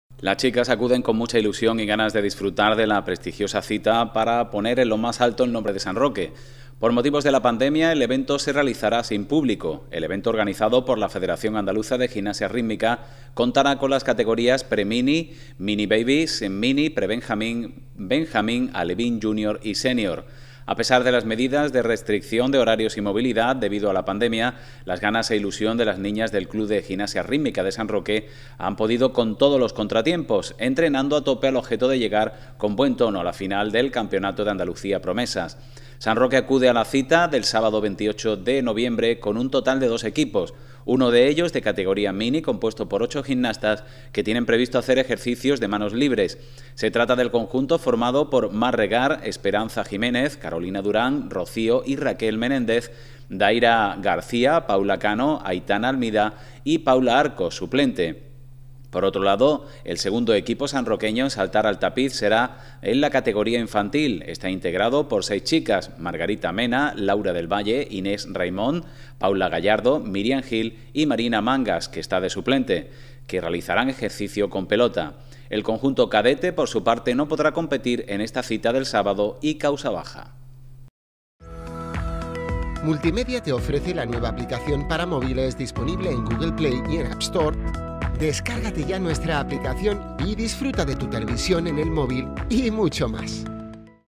NOTICIA GIMNASIA RITMICA 25 NOVIEMBRE 2020
Dos conjuntos sanroqueños asisten el sábado a la Final del Campeonato de Andalucía Promesas de Gimnasia Rítmica
Numerosa participación sanroqueña en una de las citas deportivas más relevantes de la gimnasia rítmica andaluza gracias a la participación de gimnastas de dos equipos del club que capitanea María Pérez Lobato.